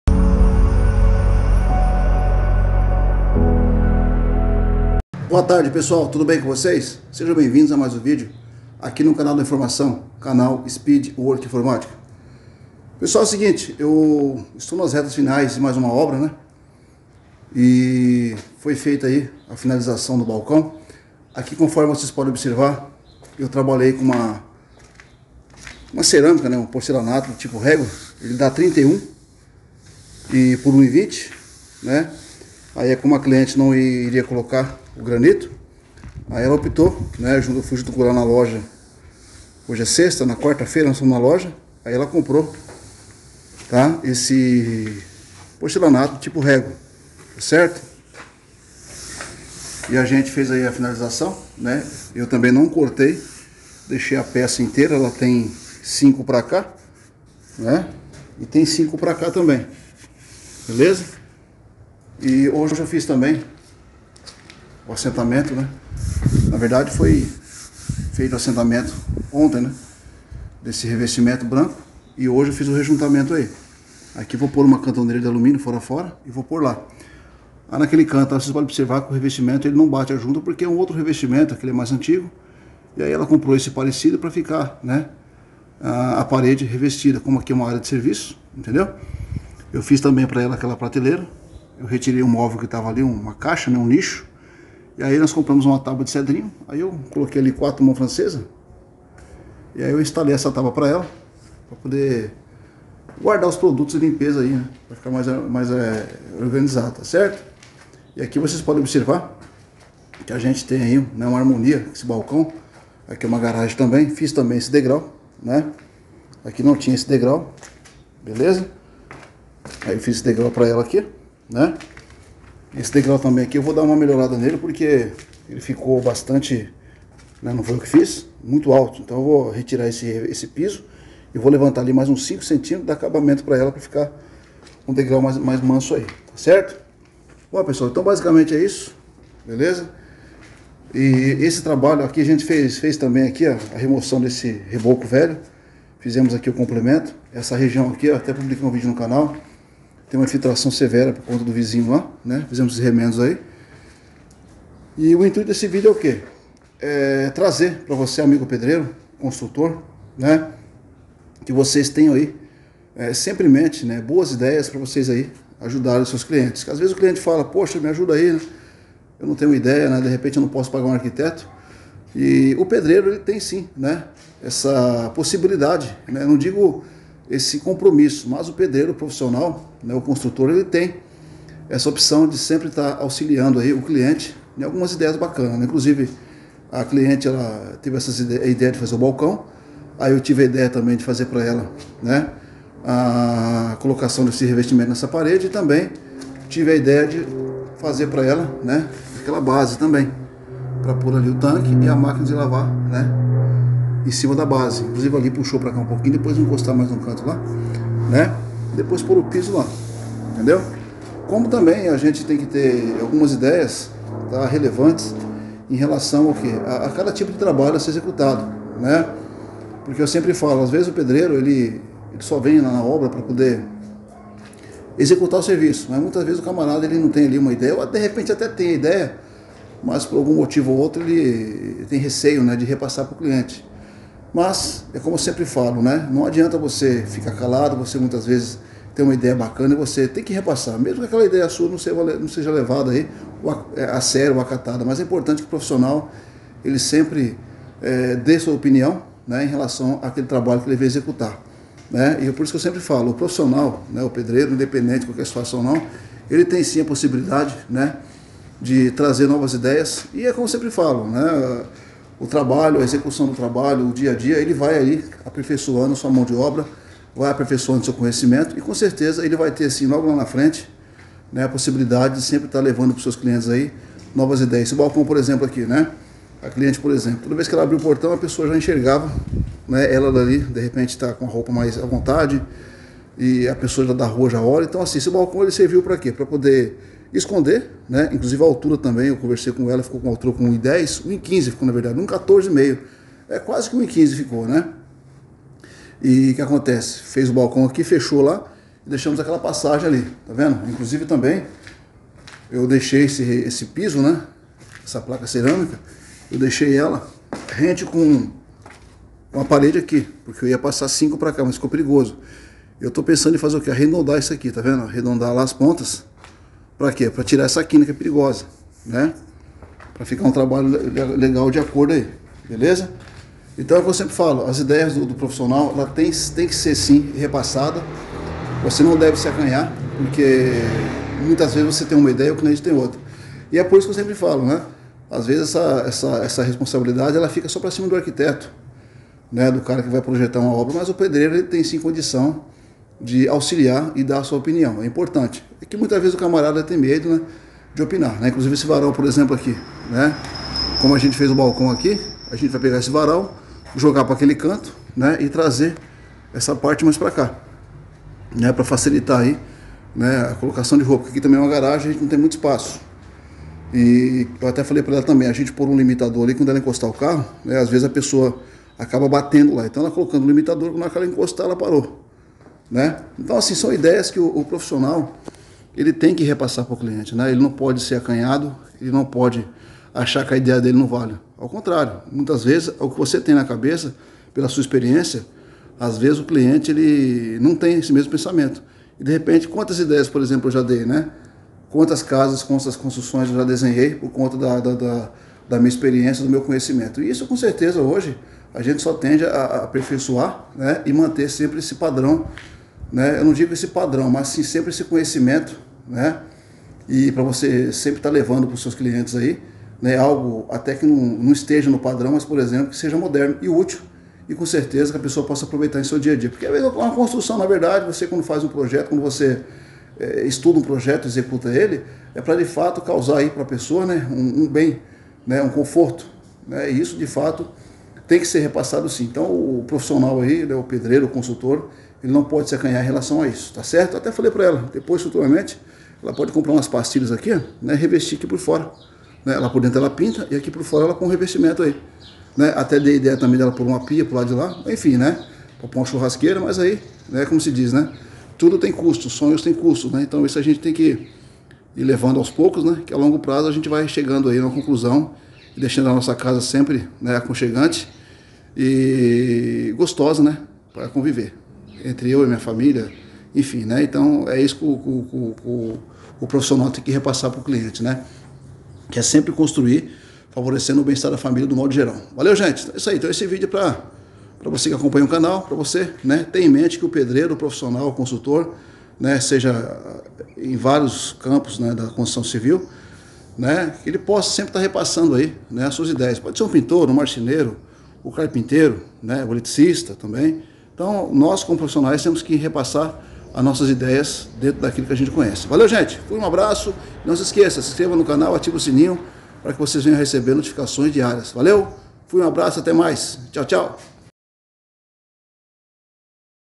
Pedreiros que dão ideias na hora de construir ou reformar terão mais sucessos em suas obras
►►Leiam◄◄

Neste vídeo vocês vão conferir na íntegra, muitas informações referentes, aquelas ideias na hora de repassar informações relevantes para seus clientes; quando há dúvidas na execução de suas obras, principalmente quando o cliente tem dificuldades de escolher o melhor projeto para a sua construção ou reforma.